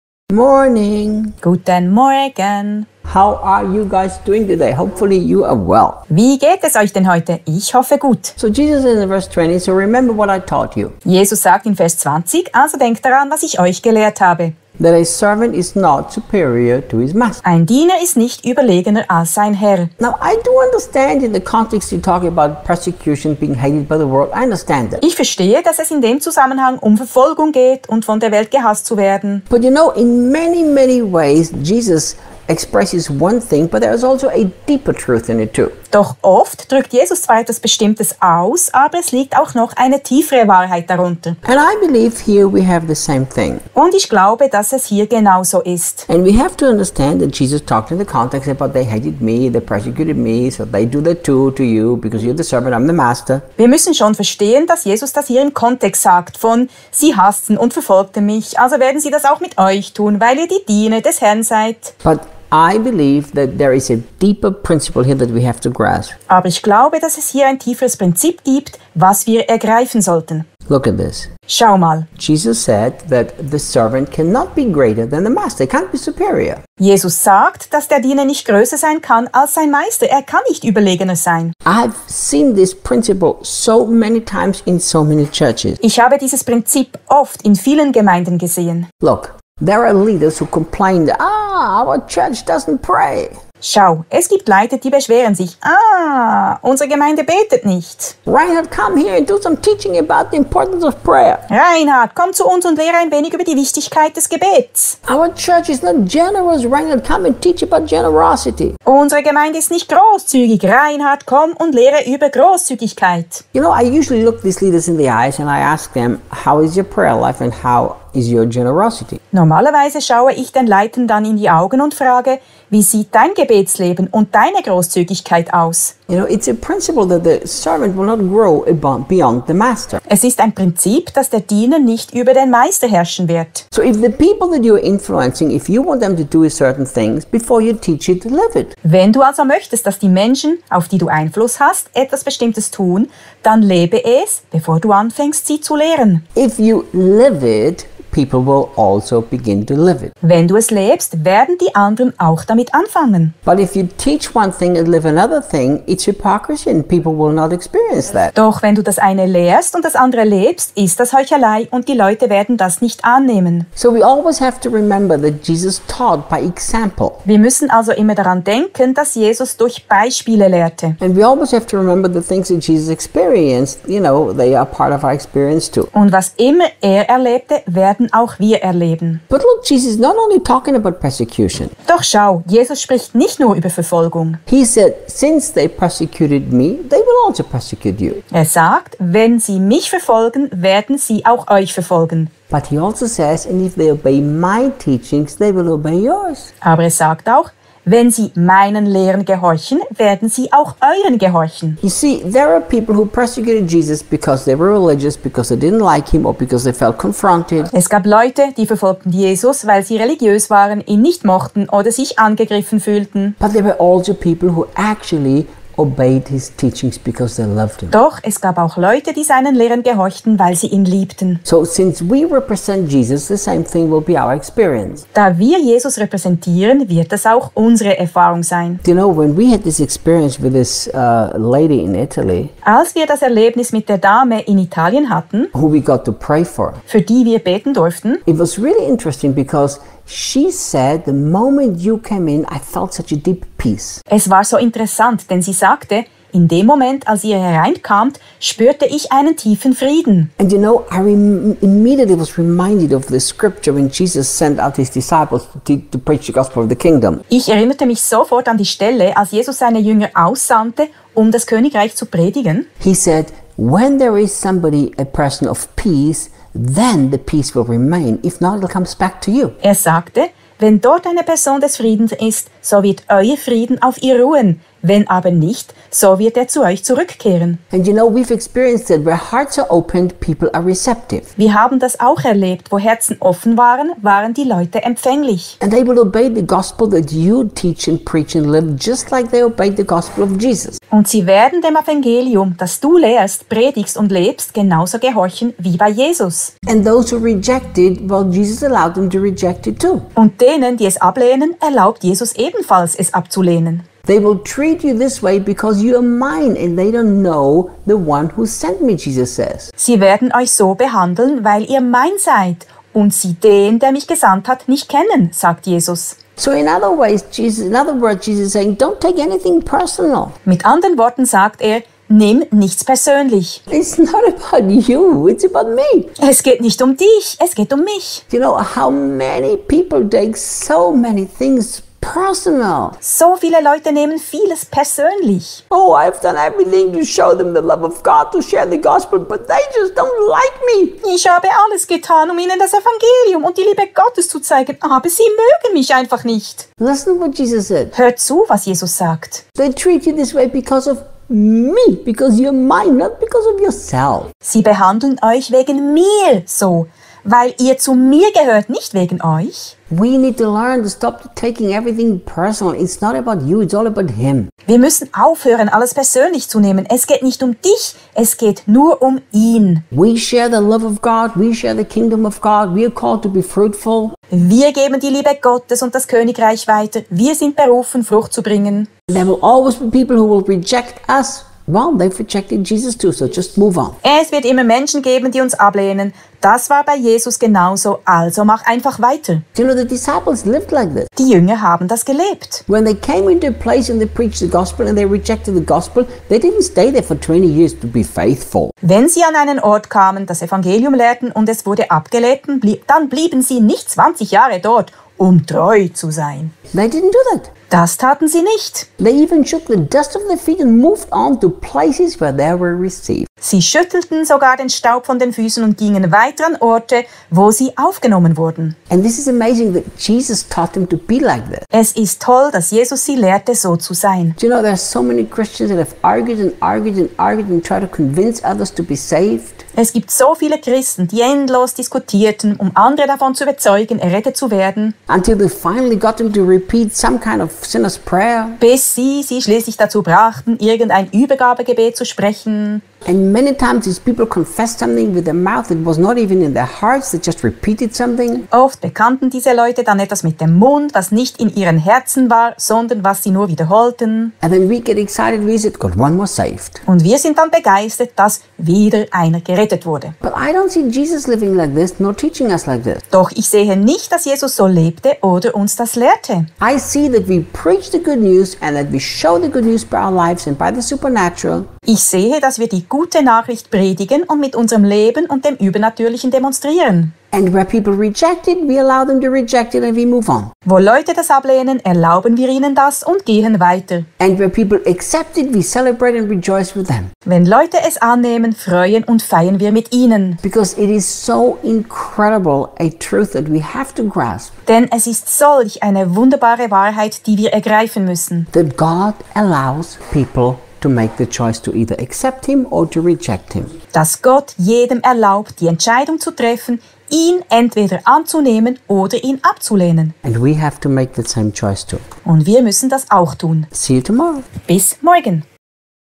Tag 16 👑 Johannes 15
Johannes 15 – Tag 16 (Verse 20 - 21)
Jesus sagt in Vers 20: „Also denkt daran, was ich euch gelehrt habe: Ein Diener ist nicht überlegener als sein Herr.“ Ich verstehe, dass es in dem Zusammenhang um Verfolgung geht und von der Welt gehasst zu werden. Doch oft drückt Jesus zwar etwas Bestimmtes aus, aber es liegt auch noch eine tiefere Wahrheit darunter und ich glaube, dass es hier genauso ist. Wir müssen schon verstehen, dass Jesus das hier im Kontext sagt von „sie hassten und verfolgten mich, also werden sie das auch mit euch tun, weil ihr die Diener des Herrn seid“, aber ich glaube, dass es hier ein tieferes Prinzip gibt, was wir ergreifen sollten.

Schau mal, Jesus sagt, dass der Diener nicht größer sein kann, als sein Meister, er kann nicht überlegener sein. Ich habe dieses Prinzip oft in vielen Gemeinden gesehen. Schau, es gibt Leiter, die beschweren sich: „Ah, unsere Gemeinde betet nicht. Reinhard komm zu uns und lehre ein wenig über die Wichtigkeit des Gebets.“ „Unsere Gemeinde ist nicht großzügig. Reinhard komm und lehre über Großzügigkeit.“ Normalerweise schaue ich den Leitern dann in die Augen und frage: „Wie sieht dein Gebetsleben und deine Großzügigkeit aus?“ Es ist ein Prinzip, dass der Diener nicht über den Meister herrschen wird. Wenn du also möchtest, dass die Menschen, auf die du Einfluss hast etwas bestimmtes tun, dann lebe es, bevor du anfängst sie zu lehren. Wenn du es lebst, werden die anderen auch damit anfangen. Doch wenn du das eine lehrst und das andere lebst, ist das Heuchelei und die Leute werden das nicht annehmen. Wir müssen also immer daran denken, dass Jesus durch Beispiele lehrte und was immer er erlebte, werden auch wir erleben.

Doch schau, Jesus spricht nicht nur über Verfolgung. Er sagt: „Wenn sie mich verfolgten, werden sie auch euch verfolgen.“, aber er sagt auch: „Wenn sie meinen Lehren gehorchen, werden sie auch euren gehorchen.“ Es gab Leute, die verfolgten Jesus, weil sie religiös waren, ihn nicht mochten oder sich angegriffen fühlten. Doch es gab auch Leute, die seinen Lehren gehorchten, weil sie ihn liebten. Da wir Jesus repräsentieren wird das auch unsere Erfahrung sein.

Als wir das Erlebnis mit der Dame in Italien hatten, für die wir beten durften. Es war so interessant, denn sie sagte: „In dem Moment, als ihr hereinkamt, spürte ich einen tiefen Frieden.“ Ich erinnerte mich sofort an die Stelle, als Jesus seine Jünger aussandte, um das Königreich zu predigen. Er sagte: „Wenn dort eine Person des Friedens ist, so wird euer Friede auf ihr ruhen, wenn aber nicht, so wird er zu euch zurückkehren.“ (Lk.10,6) Wir haben das auch erlebt. Wo Herzen offen waren, waren die Leute empfänglich. Und sie werden dem Evangelium, das du lehrst, predigst und lebst, genauso gehorchen, wie bei Jesus. Und denen, die es ablehnen, erlaubt Jesus ebenfalls, es abzulehnen.

„Sie werden euch so behandeln, weil ihr mein seid und sie den, der mich gesandt hat nicht kennen.“, sagt Jesus. Mit anderen Worten sagt er: „Nimm nichts persönlich. Es geht nicht um dich, es geht um mich.“ So viele Leute nehmen vieles persönlich. „Ich habe alles getan, um ihnen das Evangelium und die Liebe Gottes zu zeigen, aber sie mögen mich einfach nicht.“ Hör zu, was Jesus sagt: „Sie behandeln euch wegen mir so, weil ihr zu mir gehört, nicht wegen euch.“ Wir müssen aufhören, alles persönlich zu nehmen. Es geht nicht um dich, es geht nur um ihn! Wir geben die Liebe Gottes und das Königreich weiter, wir sind berufen Frucht zu bringen. Es wird immer Menschen geben, die uns ablehnen. Das war bei Jesus genauso, also mach einfach weiter.

Die Jünger haben das gelebt. Wenn sie an einen Ort kamen, das Evangelium lehrten und es wurde abgelehnt, dann blieben sie nicht 20 Jahre dort, um treu zu sein. Das taten sie nicht. Sie schüttelten sogar den Staub von den Füßen und gingen weiter an Orte, wo sie aufgenommen wurden. Es ist toll, dass Jesus sie lehrte, so zu sein. Es gibt so viele Christen, die endlos diskutierten, um andere davon zu überzeugen, errettet zu werden, bis sie sie schließlich dazu brachten, irgendein Übergabegebet zu sprechen. Oft bekannten diese Leute dann etwas mit dem Mund, was nicht in ihren Herzen war, sondern was sie nur wiederholten. Und wir sind dann begeistert, dass wieder einer gerettet wurde.

Doch ich sehe nicht, dass Jesus so lebte oder uns das lehrte. Ich sehe, dass wir die Gute Nachricht predigen und mit unserem Leben und dem Übernatürlichen demonstrieren. Wo Leute das ablehnen, erlauben wir ihnen das und gehen weiter. Wenn Leute es annehmen, freuen und feiern wir mit ihnen. Denn es ist solch eine wunderbare Wahrheit, die wir ergreifen müssen, dass Gott jedem erlaubt die Entscheidung zu treffen, ihn entweder anzunehmen oder ihn abzulehnen. Und wir müssen das auch tun.

n euch so behandeln, weil ihr mein seid und sie den, der mich gesandt hat nicht kennen.

Übersetzung: Mary
Voice: Carole
Bearbeitung: Jeanne / Yuri